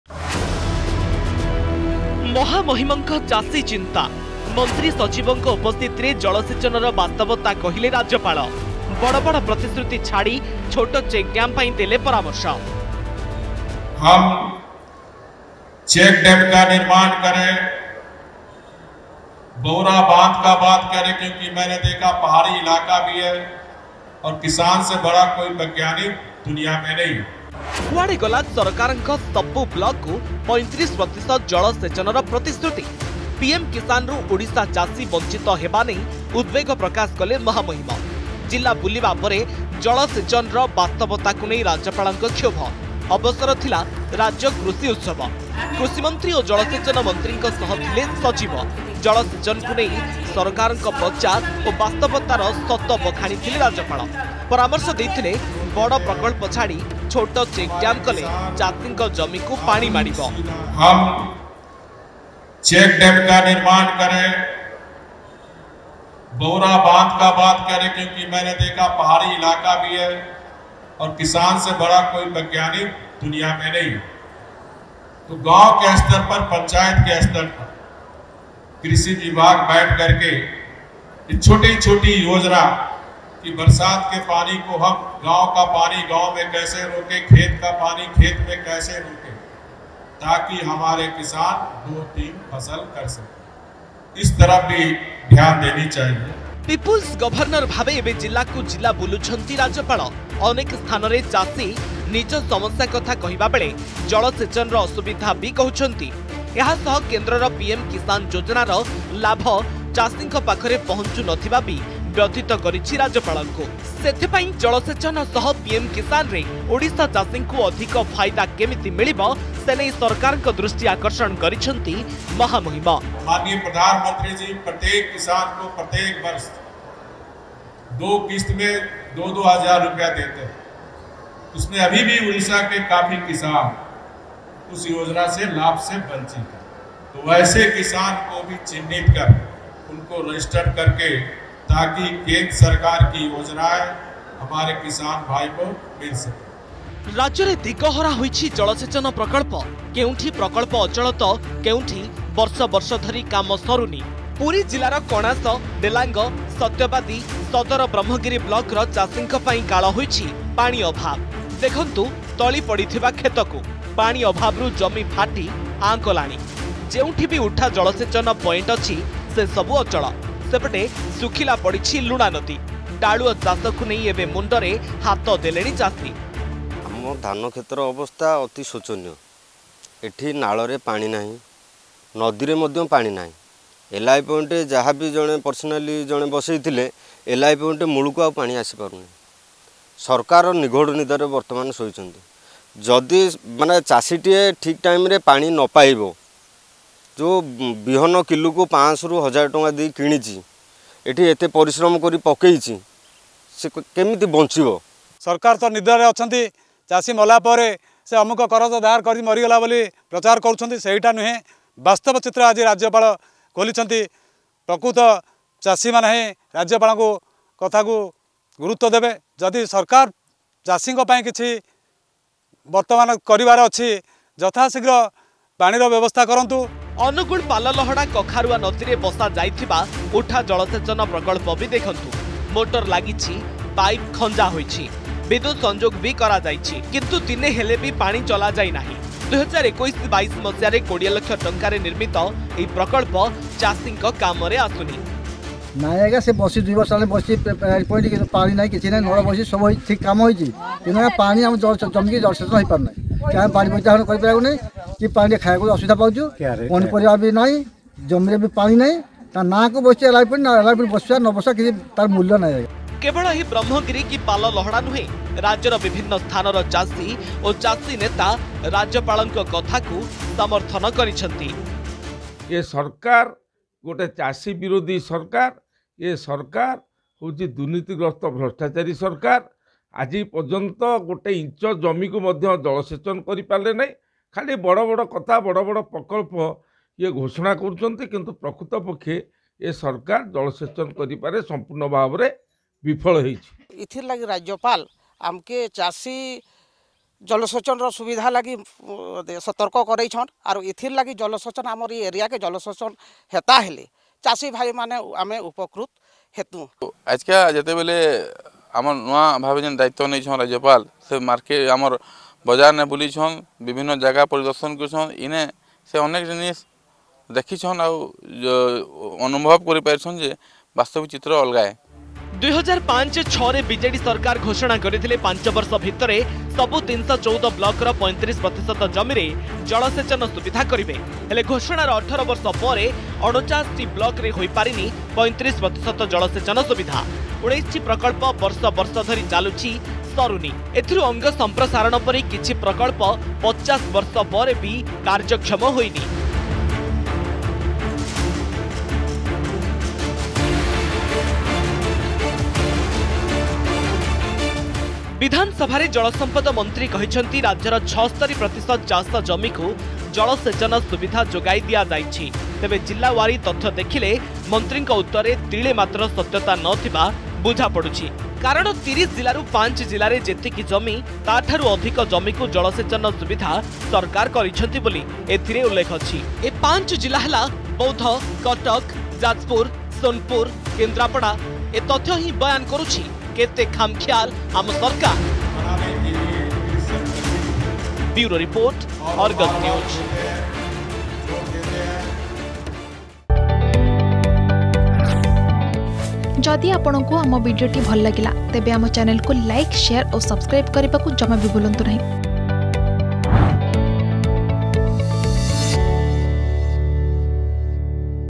ଓଡ଼ିଶା ଚାଷୀଙ୍କୁ ନେଇ ରାଜ୍ୟପାଳ ରଘୁବର ଦାସଙ୍କ ଚିନ୍ତା || Raghubar Das || Odisha
ଓଡ଼ିଶା ଚାଷୀଙ୍କୁ ନେଇ ରାଜ୍ୟପାଳ ରଘୁବର ଦାସଙ୍କ ଚିନ୍ତା । ମନ୍ତ୍ରୀ ସଚିବଙ୍କ ଉପସ୍ଥିତିରେ ଜଳସେଚନର ବାସ୍ତବତା କହିଲେ ରାଜ୍ୟପାଳ ରଘୁବର ଦାସ । କୁଆଡ଼େ ଗଲା ସରକାରଙ୍କ ସବୁ ବ୍ଲକକୁ 35 ପ୍ରତିଶତ ଜଳସେଚନର ପ୍ରଚାର ?